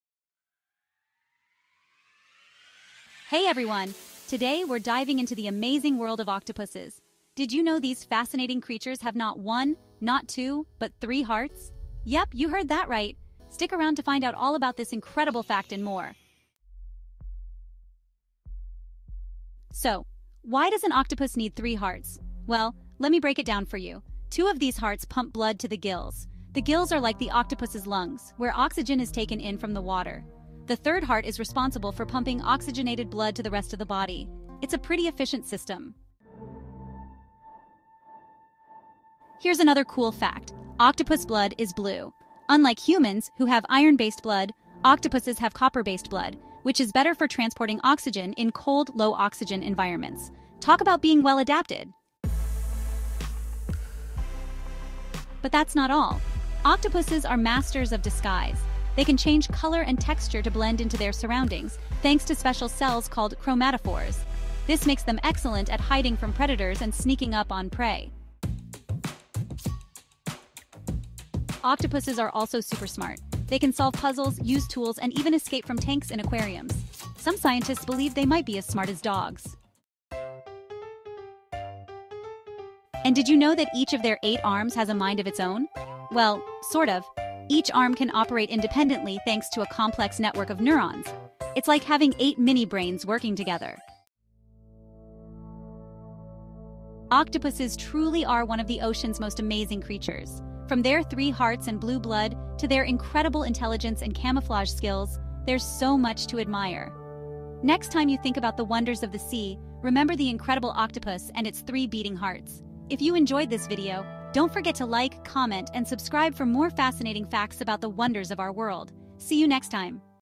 Why Do Octopuses Have Three Hearts? Discover the Secrets!
🌊 Discover the Amazing Octopus with Three Hearts! 🌊

Have you ever wondered why octopuses have three hearts? Dive into the fascinating world of these incredible sea creatures in our latest video! 🐙💙

In this video, we'll explore:

The unique anatomy of octopuses and the role of their three hearts.
How their blue blood helps them survive in deep ocean waters.
Amazing facts about their intelligence, camouflage abilities, and more!
Join us on this underwater adventure and learn why octopuses are one of the ocean’s most extraordinary inhabitants. Perfect for marine biology enthusiasts, animal lovers, and anyone curious about the wonders of the sea.